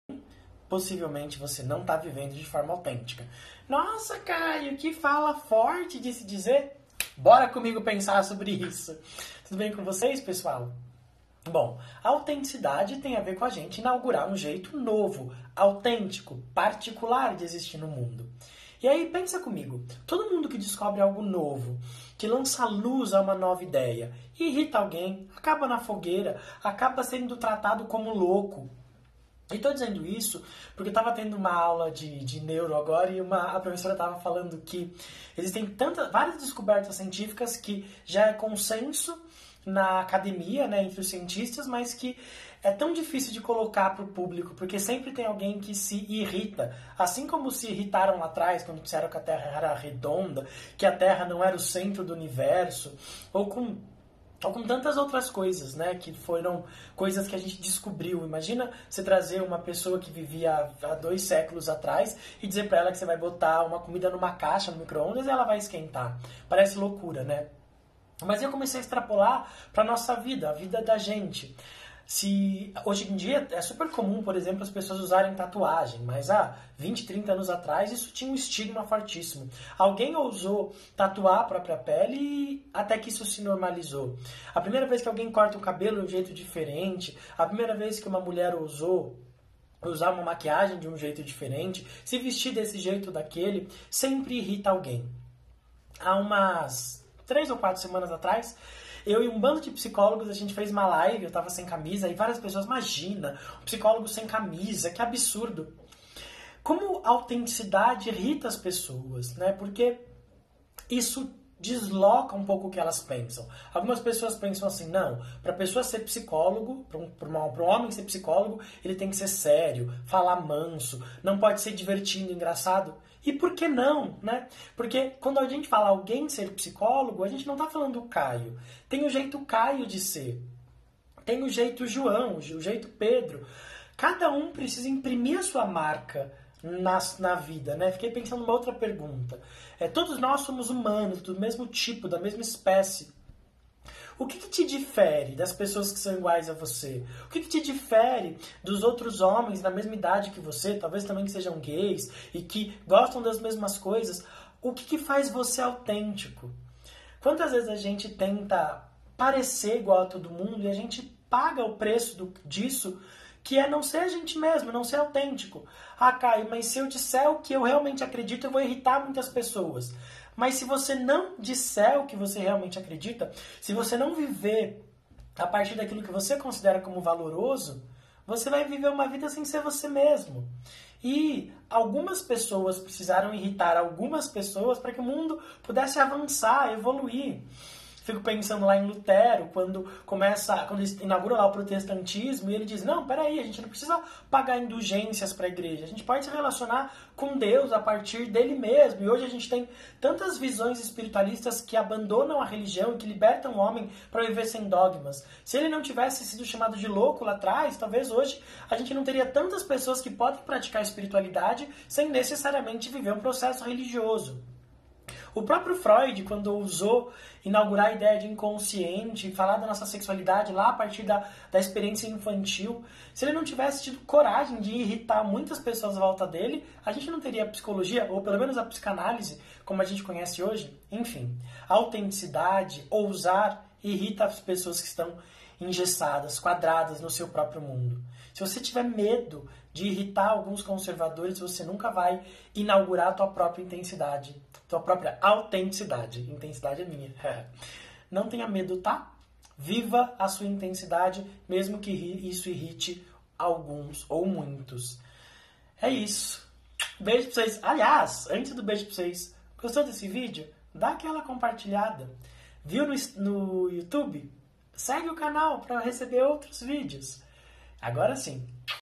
Inaugurar Algo Novo Irrita Algumas Pessoas.
Ser autêntico pode irritar algumas pessoas, pode incomodar e gerar sim alguns desconfortos...
Mas é muito pior passar uma vida sem inaugurar quem a gente realmente é!

Quer fazer terapia comigo? Mande uma msg